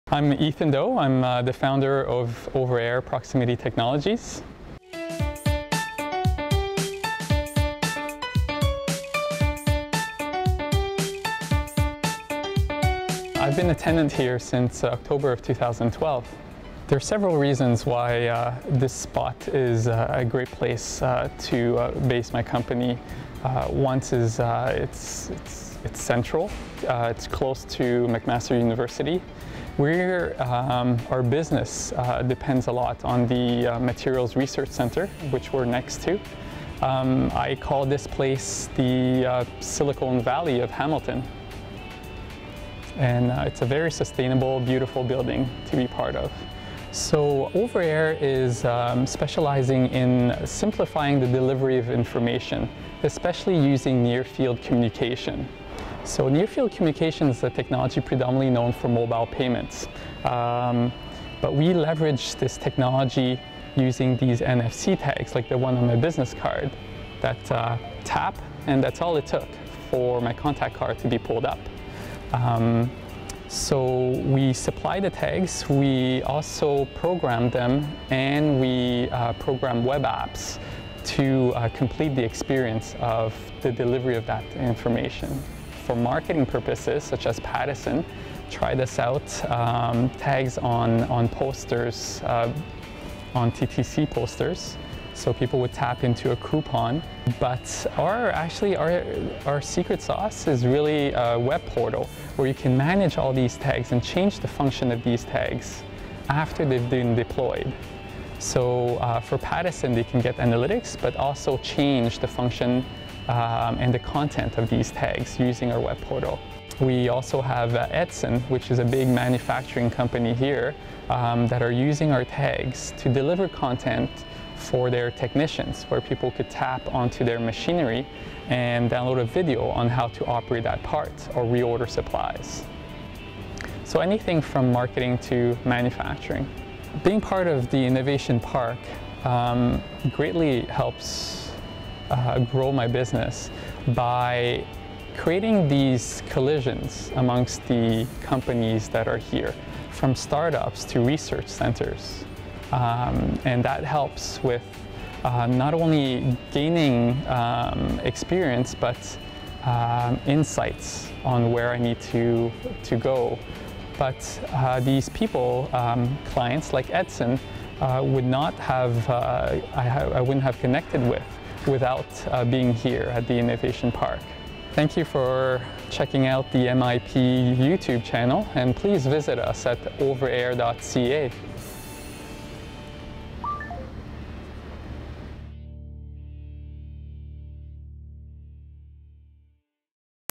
OverAir Proximity Technologies: NFC Marketing, Intelligence, Technology
Ethan Do, founder of OverAir Proximity Technologies explains how Near Field Communication technology can help your business, and the benefits of co-locating at McMaster Innovation Park.